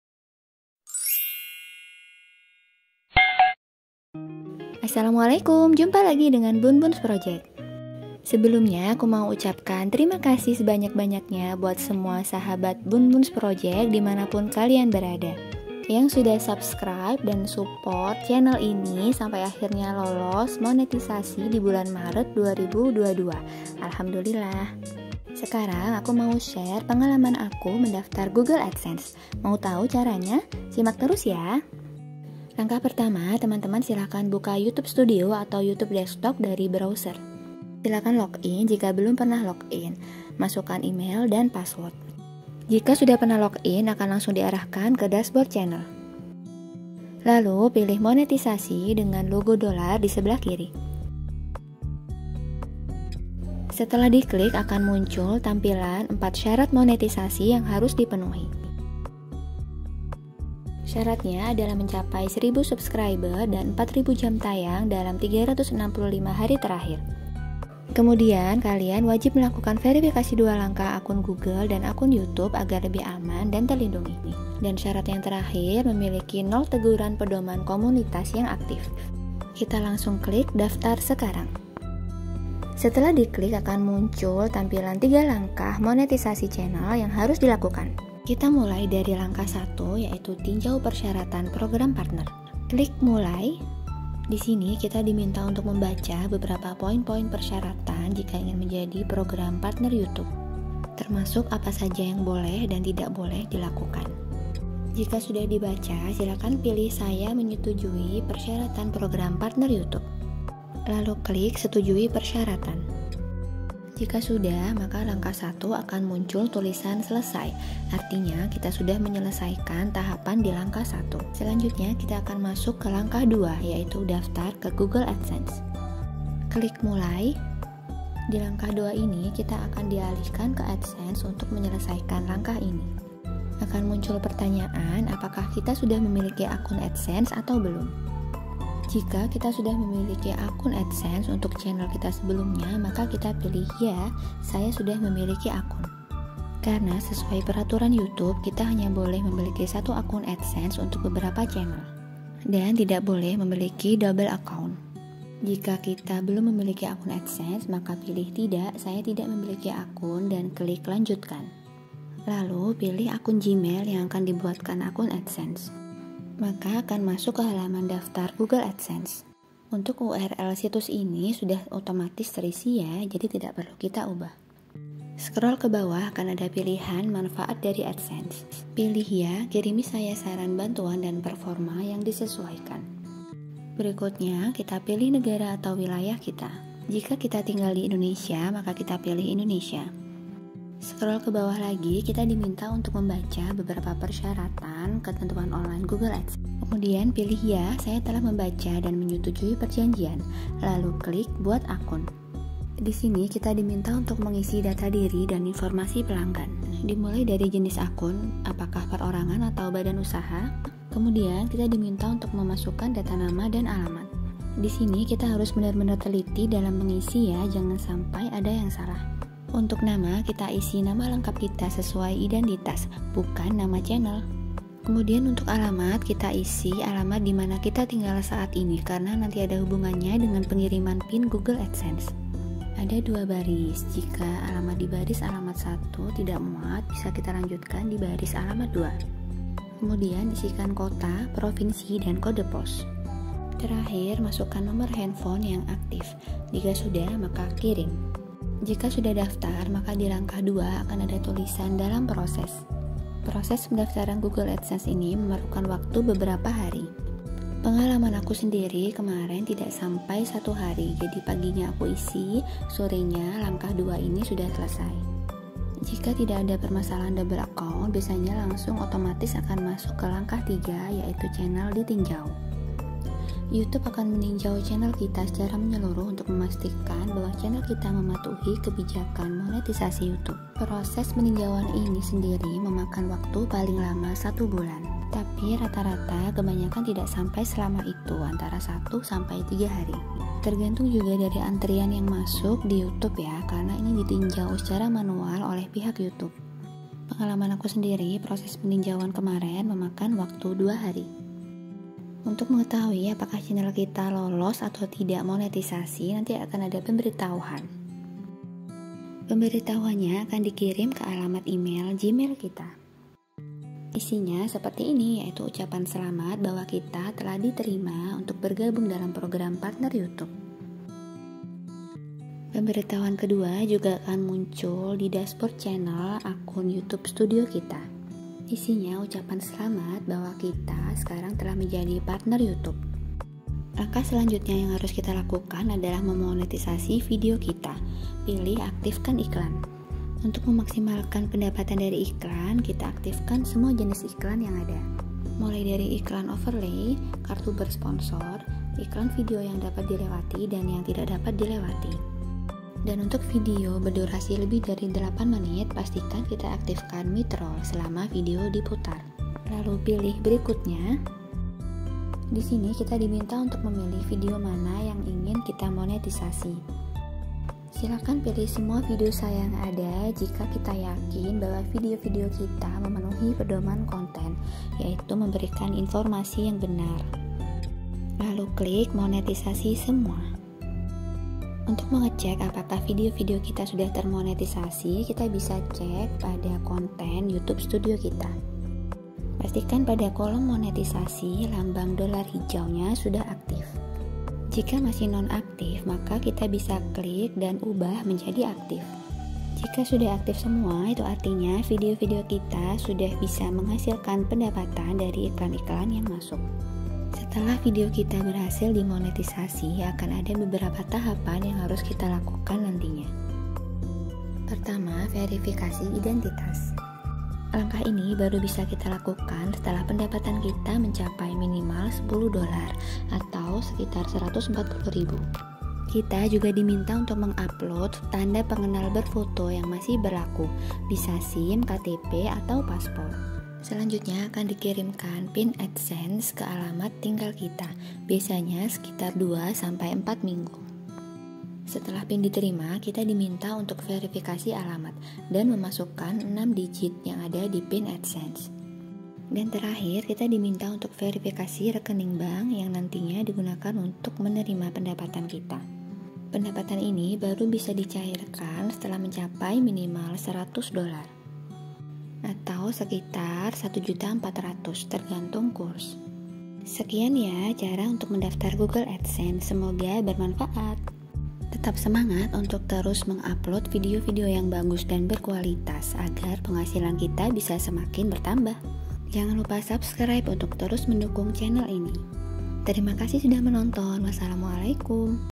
Cara Daftar Google Adsense 2022 Lewat HP Android
Video tentang tutorial cara daftar google Adsense 2022 Lewat hp android. Berdasarkan pengalaman aku lolos monetisasi dalam waktu 2-3 hari.

Dalam video ini juga dijelaskan syarat monetisasi (syarat program partner YouTube),  tutorial daftar google Adsense, hingga peninjauan. selain itu juga dibahas verifikasi identias, pengiriman pin Adsense dan verifikasi alamat serta rekening bank.

Pemusik: @iksonmusic